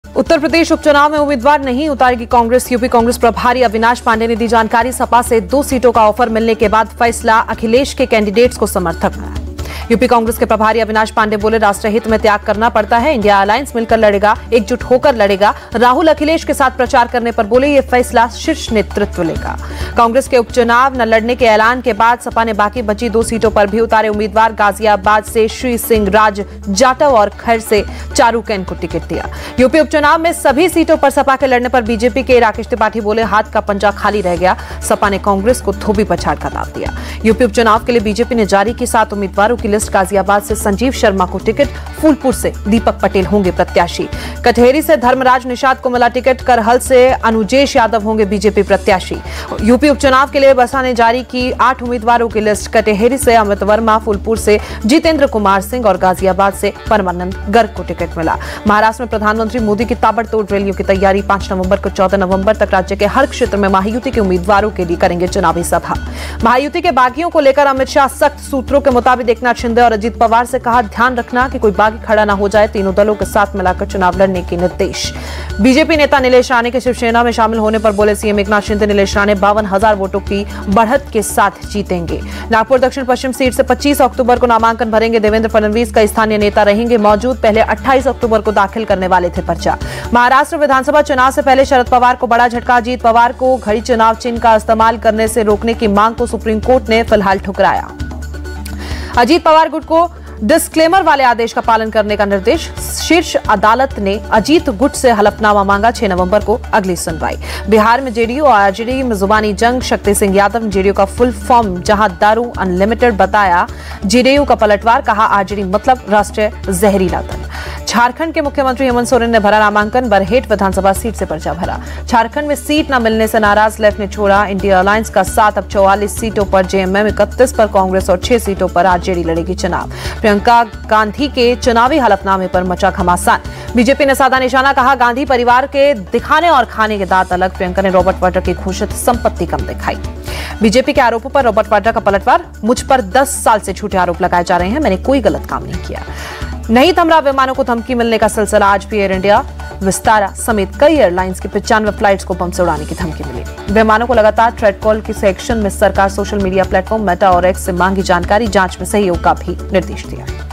UP By-Elections 2024: UP By-Elections में उम्मीदवार नहीं उतारेगी Congress | UP News | Speed News
UP By Election 2024 Update: इंडिया गठबंधन में समाजवादी पार्टी के मुखिया अखिलेश यादव के दांव के आगे कांग्रेस चित हो गई या कांग्रेस ने लड़ने का मन ही नहीं बनाया.

UP By Election 2024 Update: Congress got worried in front of Samajwadi Party chief Akhilesh Yadav's move in India alliance or did Congress not want to fight?